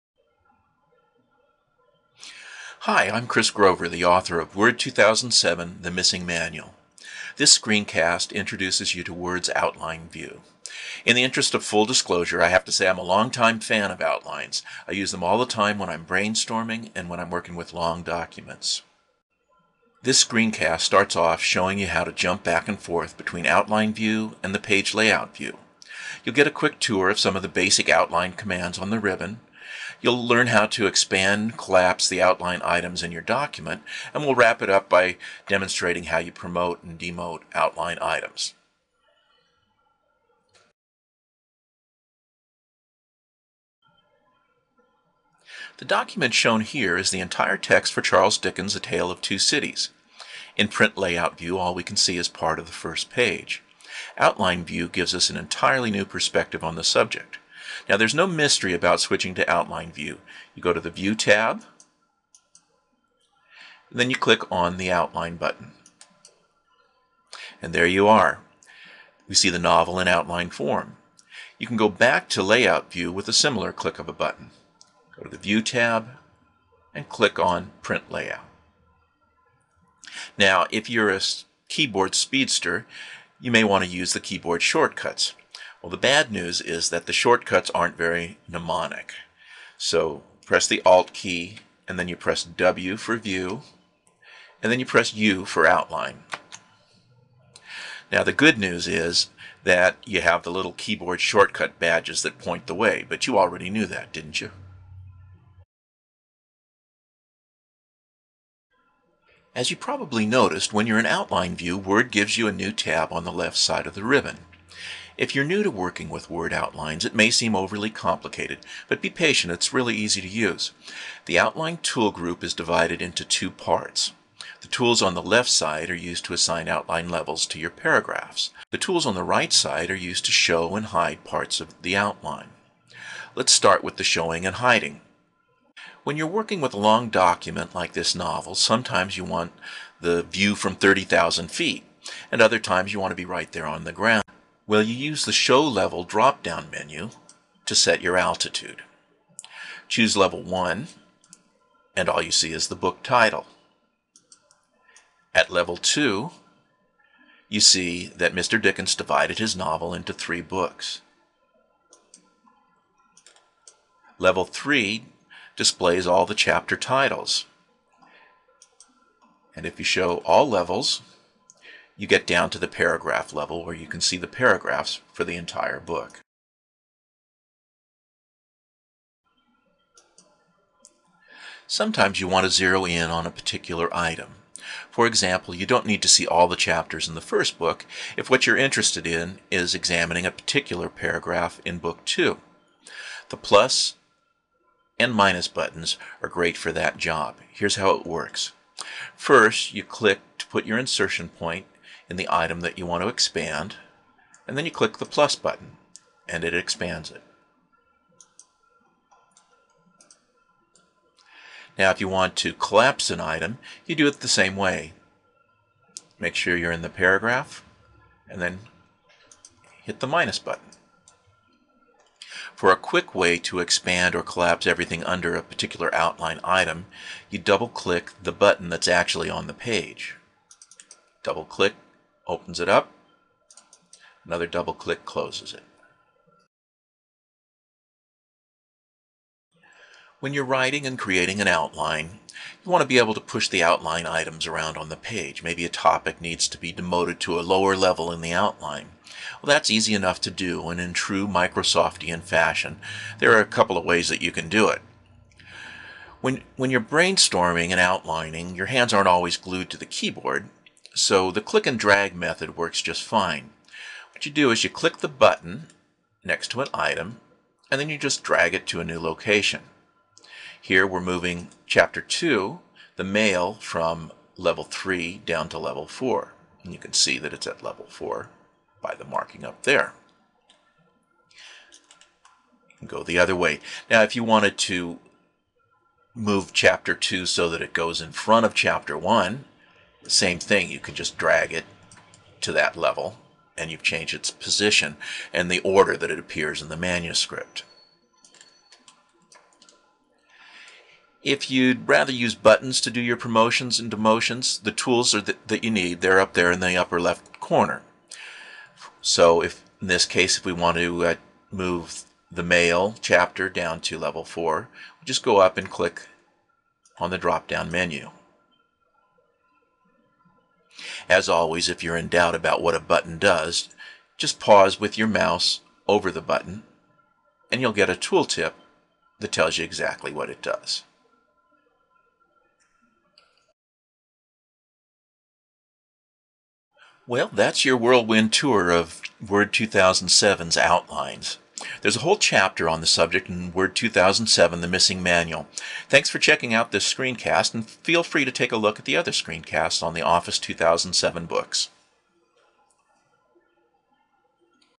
Working With Outlines in Word 2007
missingmanuals.com - This screencast from Chris Grover, author of Word 2007: The Missing Manual, guides you through working with outlines in a Word 2007 document.